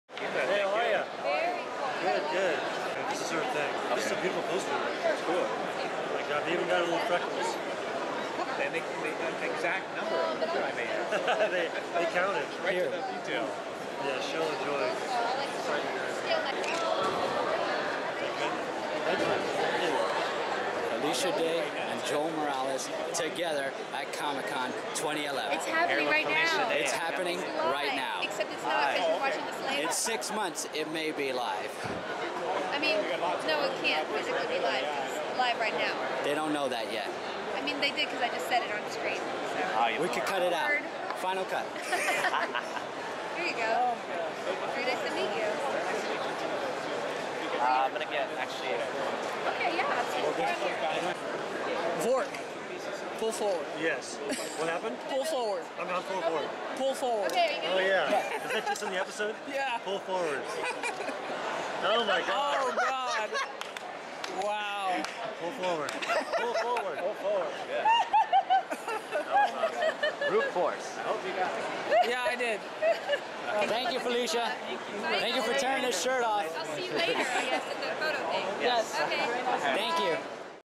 Felicia Day Rips Open My Shirt!
Video of me meeting Jeff Lewis "Vork" & Felicia Day.